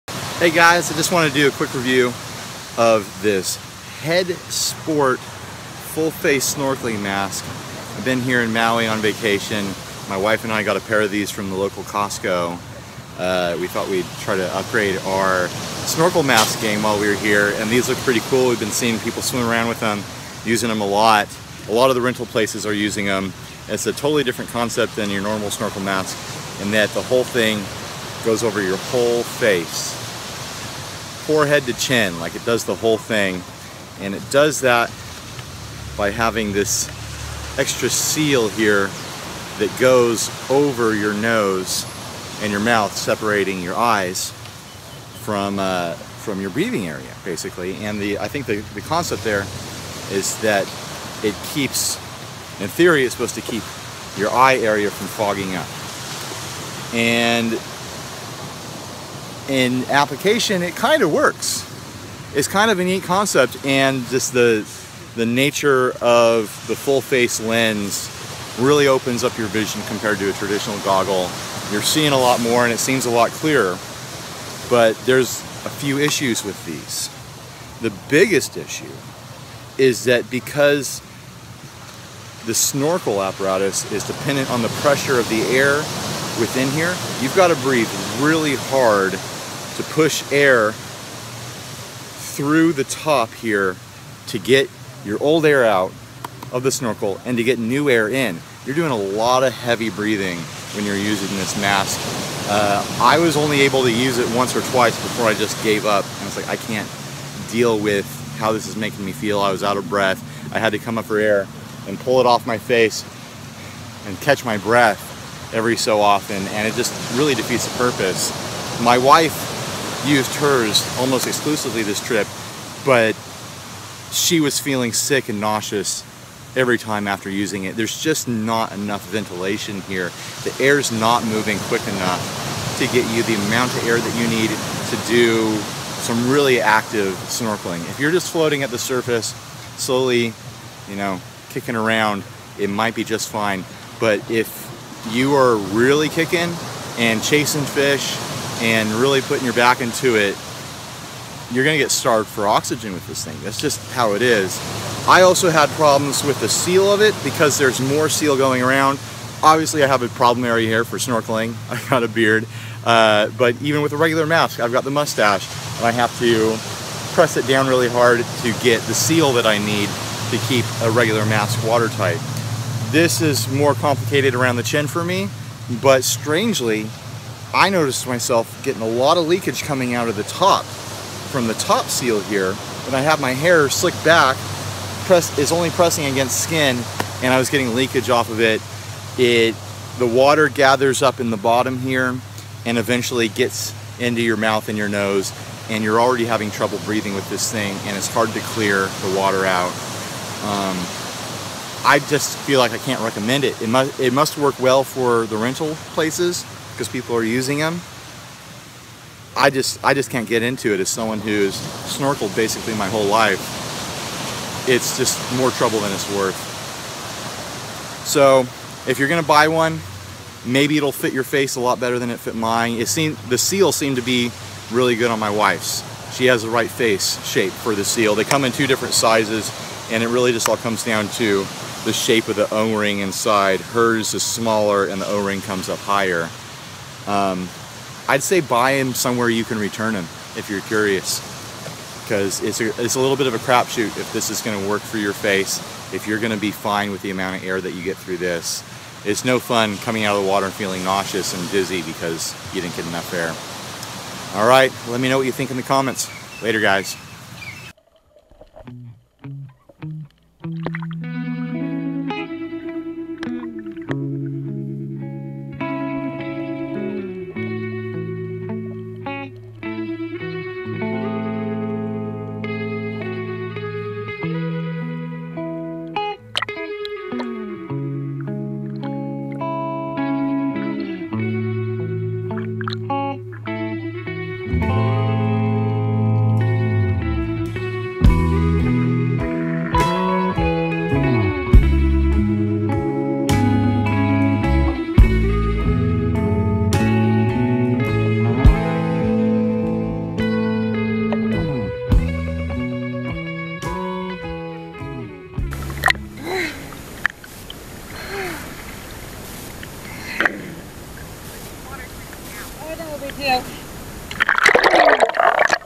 Head Sport - full face snorkel mask review
Ryan reviews his experience with the Head Sport Full Face Mask that he bought at costco while in Maui, Hawaii

All footage shot with this camera